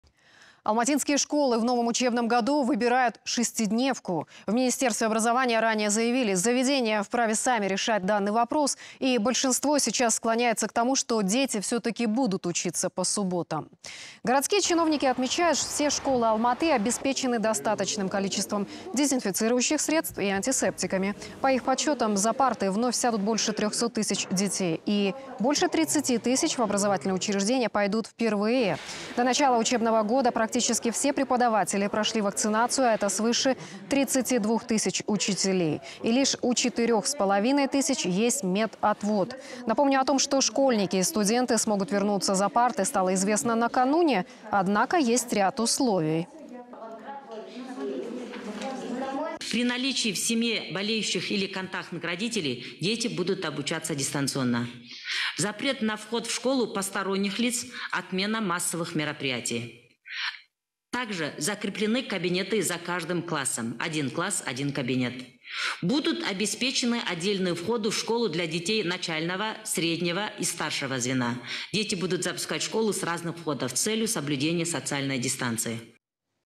Алматинские школьники будут учиться по субботам
Алматинские школы в новом учебном году выбирают шестидневку. В Министерстве образования ранее заявили, заведения вправе сами решать данный вопрос. И большинство сейчас склоняется к тому, что дети все-таки будут учиться по субботам.